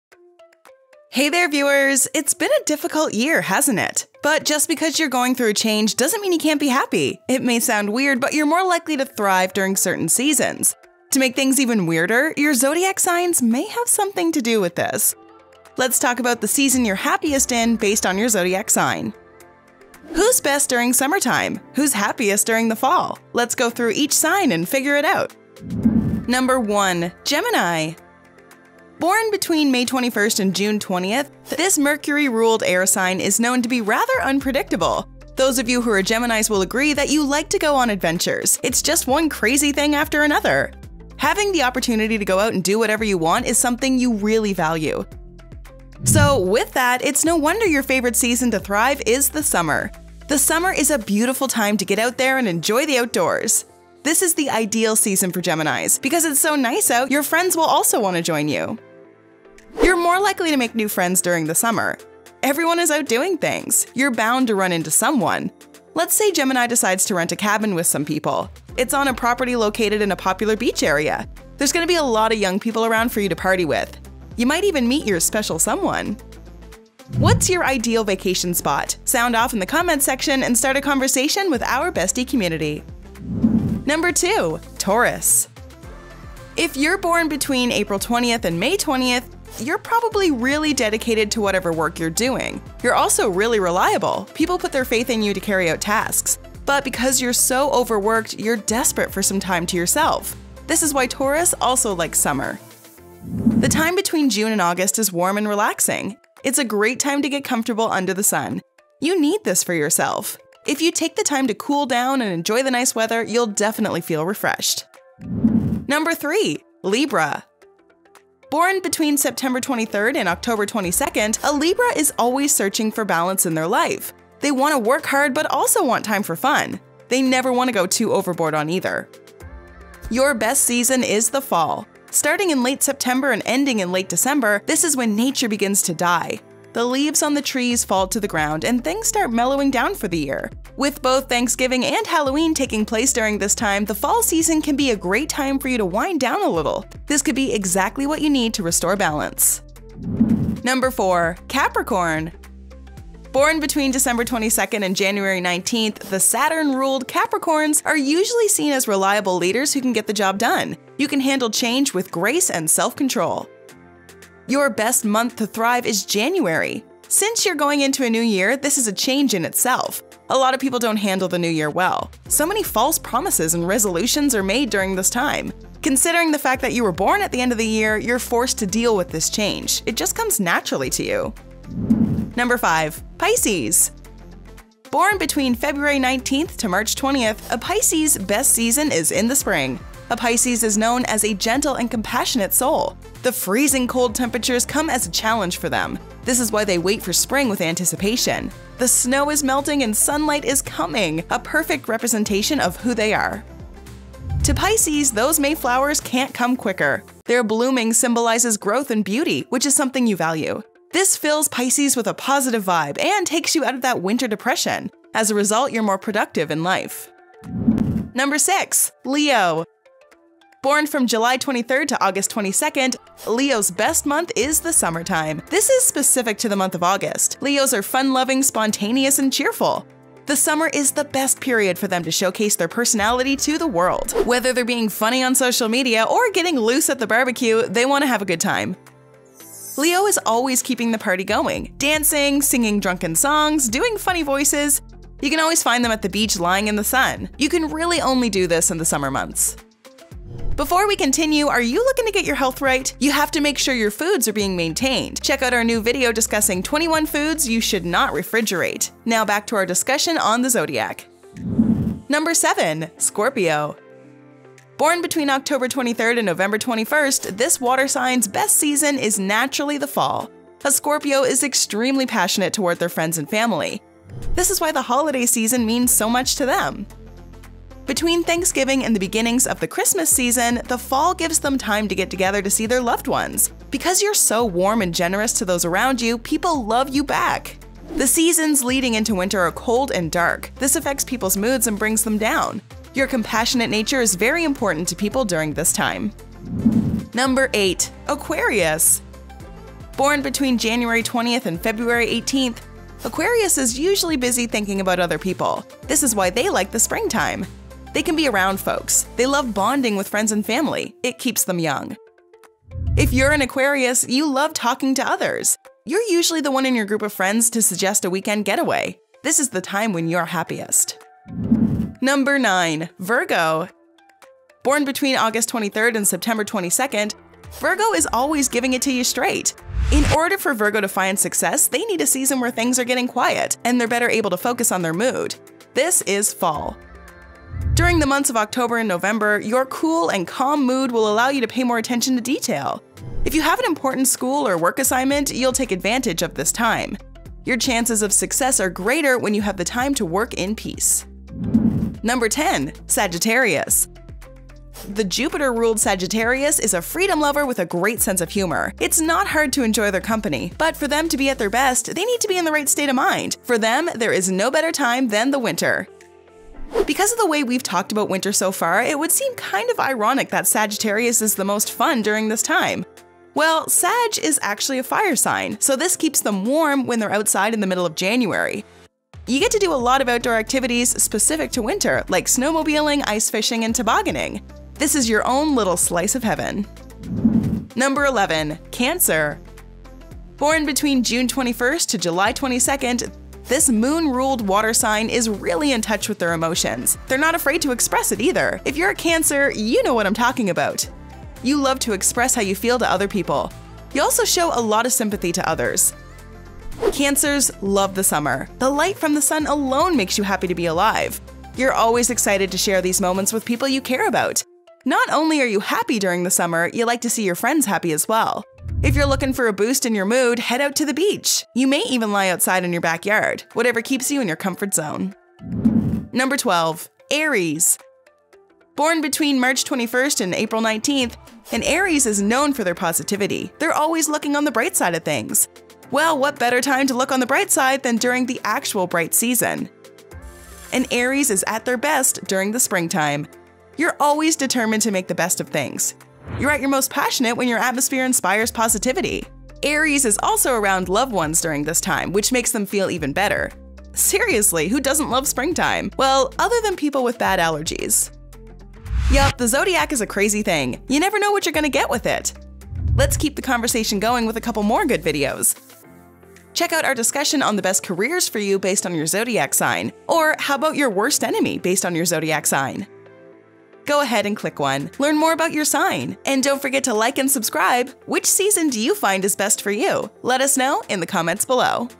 The Season You're Happiest In Based On Your Zodiac Sign
Who’s best during summer time? Who’s happiest during the fall? Let’s go through each sign and figure it out.

Timestamps:
Intro - 0:00
Gemini - 0:26
Taurus - 1:23
Libra - 1:56
Capricorn - 2:32
Pisces - 3:02
Leo - 3:38
Scorpio - 4:22
Aquarius - 4:57
Virgo - 5:22
Sagittarius - 5:52
Cancer - 6:28
Aries - 7:07

Summary:
1. Gemini
Born between May 21st and June 20th, this Mercury-ruled air sign is known to be rather unpredictable. Those of you who are Gemini’s will agree that you like to go on adventures. It’s just one crazy thing after another. Having the opportunity to go out and do whatever you want is something you really value.

2. Taurus
If you’re born between April 20th and May 20th, you’re probably really dedicated to whatever work you’re doing. You’re also really reliable. People put their faith in you to carry out tasks. But  because you’re so overworked, you’re desperate for some time to yourself. This is why Taurus also likes summer.

3. Libra
Born between September 23rd and October 22nd, a Libra is always searching for balance in their life. They want to work hard but also want time for fun. They never want to go too overboard on either.

4. Capricorn
Born between December 22nd and January 19th, the Saturn-ruled Capricorns are usually seen as reliable leaders who can get the job done. You handle change with grace and self-control.

5. Pisces
Born between February 19th to March 20th, a Pisces' best season is in the spring A Pisces is known as a gently and compassionate soul. The freezing cold temperatures come as a challenge for them. This is why they wait for spring with anticipation. The snow is melting and sun light is coming, a perfect representation of who they are.